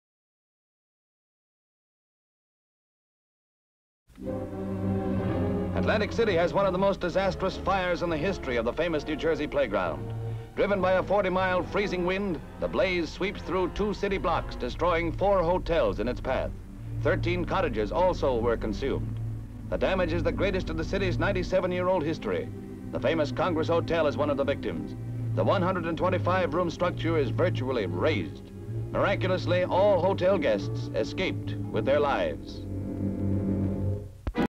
Firefighters extinguish blaze spanning 2 city blocks, 4 hotels, and 13 cottages i...HD Stock Footage
Firefighters extinguish blaze spanning 2 city blocks, 4 hotels, and 13 cottages in Atlantic City, New Jersey.

Firefighters use water hoses on buildings in Atlantic City following a large fire that spanned 2 city blocks, destroying 4 hotels and 13 cottages. Scenes of demolished buildings. A sign for the Congress Hotel half burned and laying on the ground. View of the steps of the Congress Hotel which is all that remains of the completely destroyed building. Firemen working. Scenes of wreckage. Location: Atlantic City New Jersey. Date: January 7, 1952.

57,000+ broadcast-quality historic clips for immediate download.